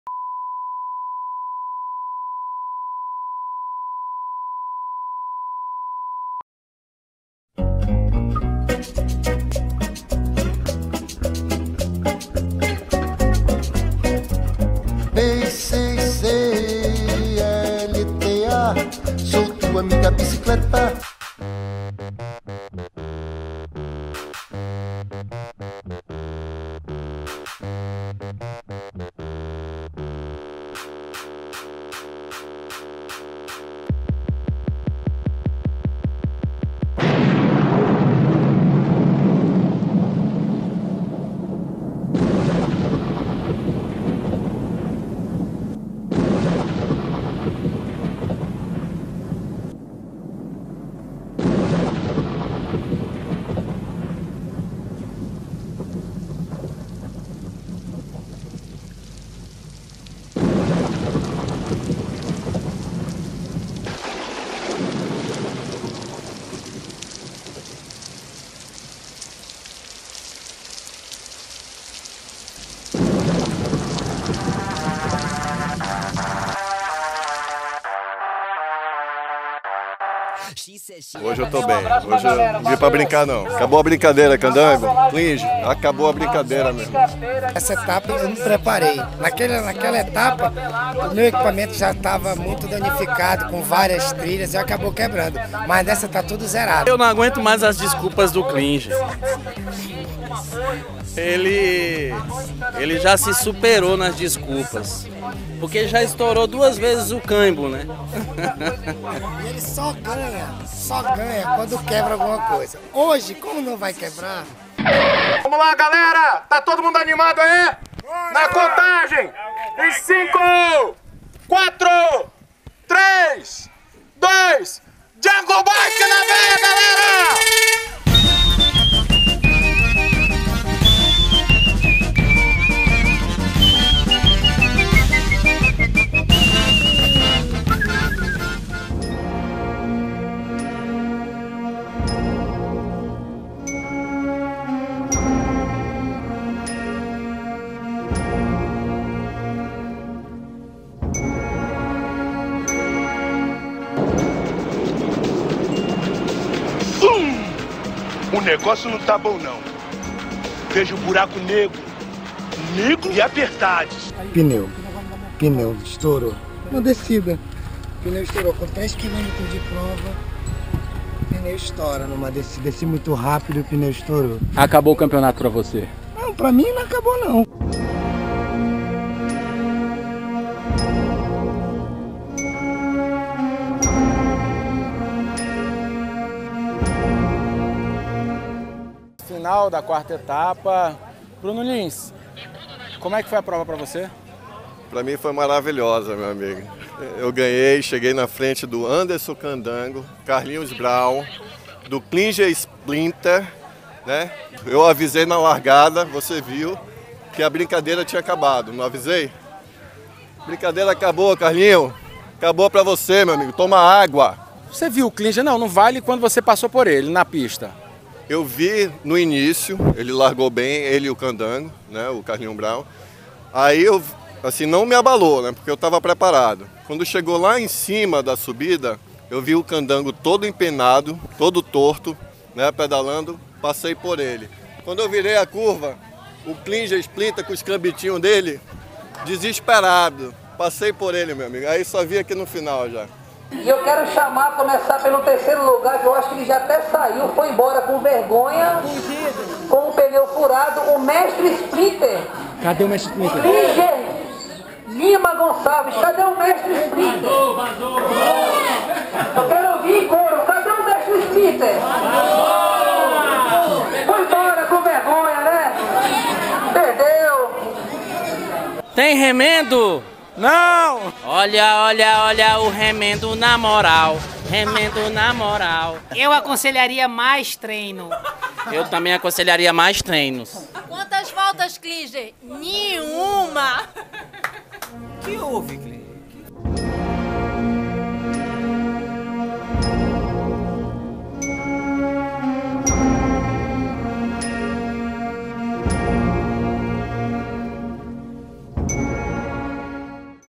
Desafio Quarta Etapa: o enterro de Mestre Spliter.
Penúltimo Capítulo do desafio Anderson Candango X Bruno Lins X Klinger Gonçalves.